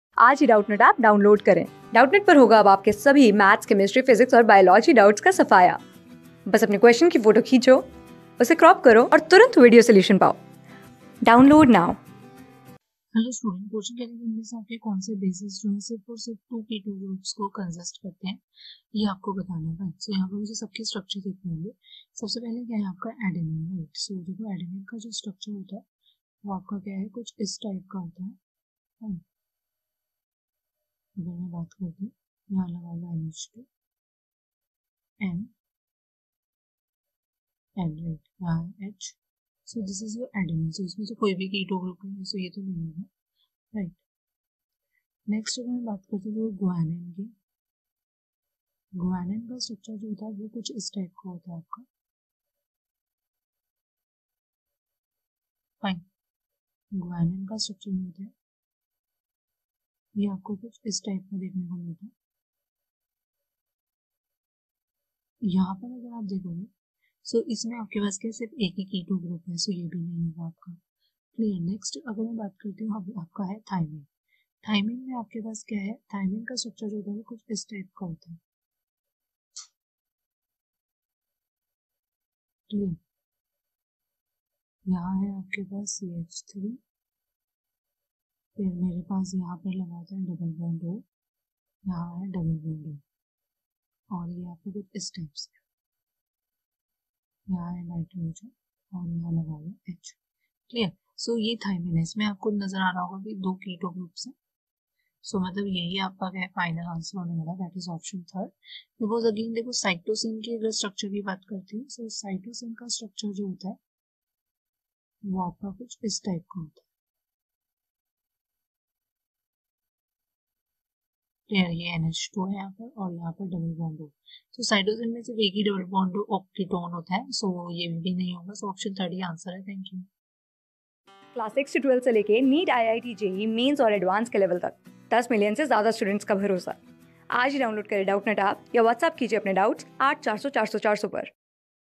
Which of the following bases contains two keto groups ? | CLASS 12 | SAMPLE PAPER 4 | CHEMISTRY ...
Which of the following bases contains two keto groups ?
Class: 12
Subject: CHEMISTRY
Chapter: SAMPLE PAPER 4
Board:CBSE

👉 Have Any Query? Ask Us.